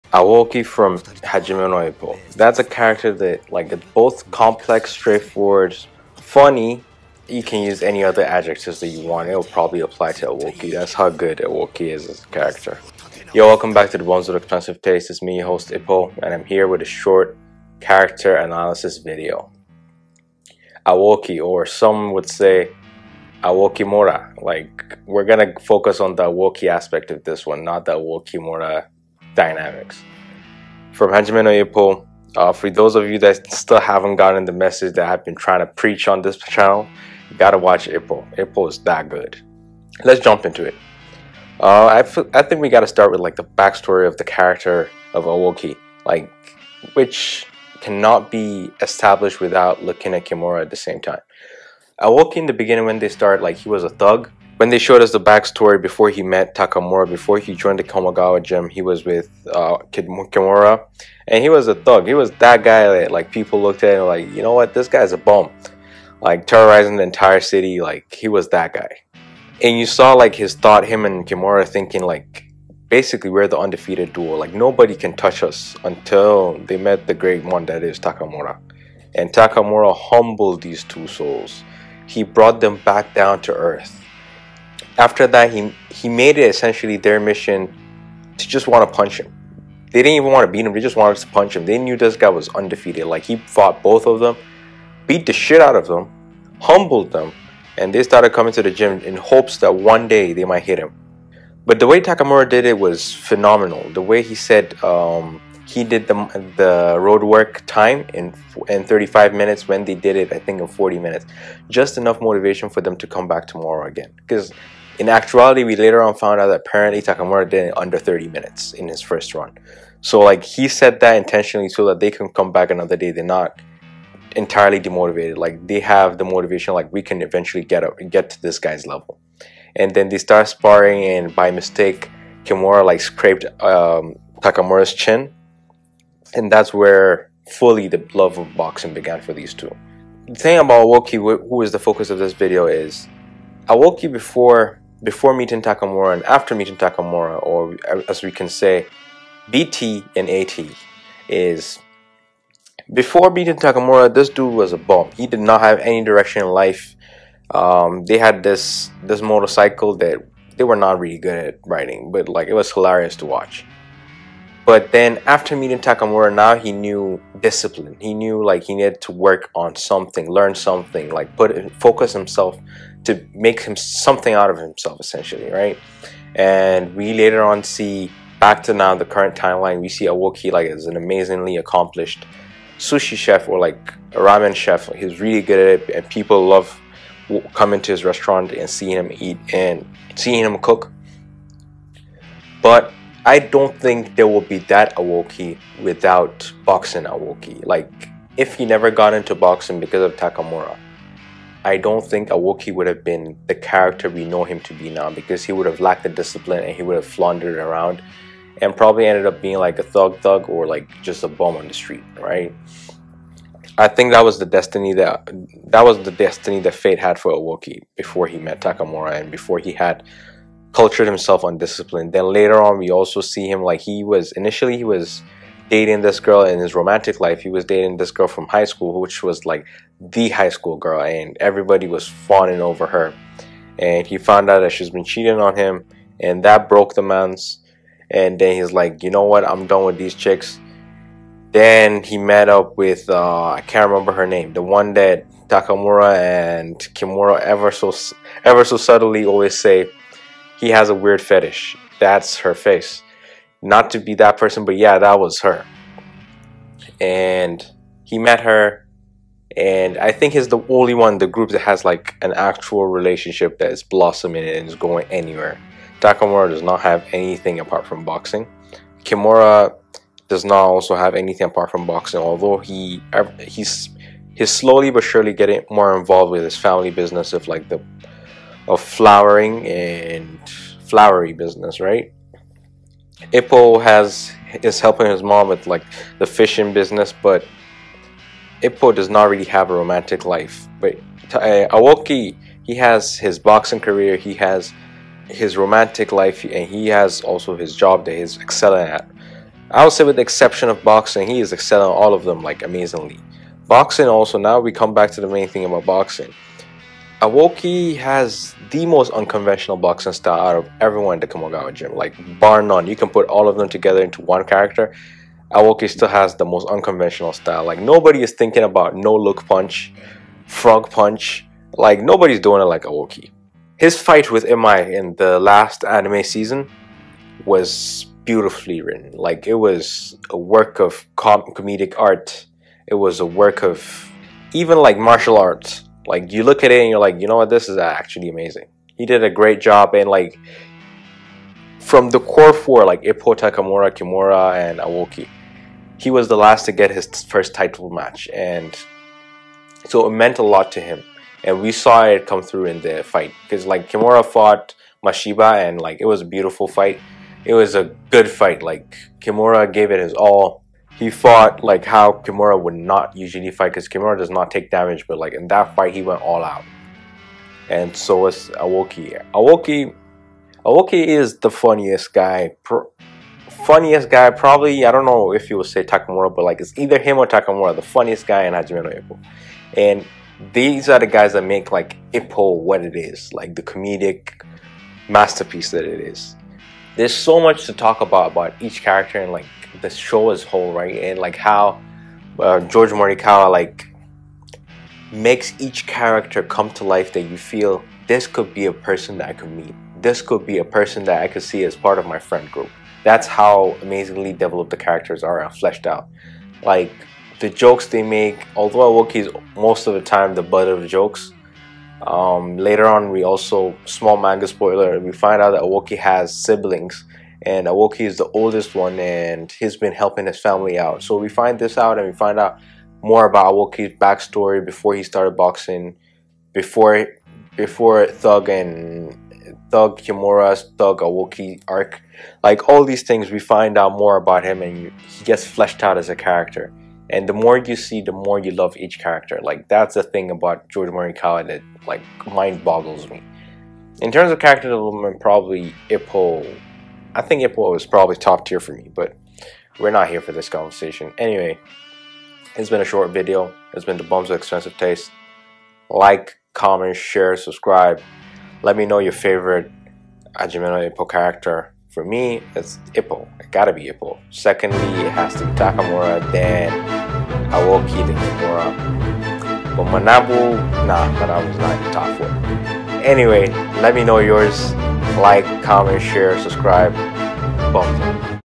Aoki | Hajime no Ippo Character Analysis
Ippo is one of the greatest funniest anime out there i recommend you watch it.

0:00 Intro
0:40 Backstory
2:58 well rounded life (romance, career etc)
5:03 Boxing 🥊 career
6:10 funniest/ character development
7:32 ending

Also thank you to our new and old viewers and subscribers :)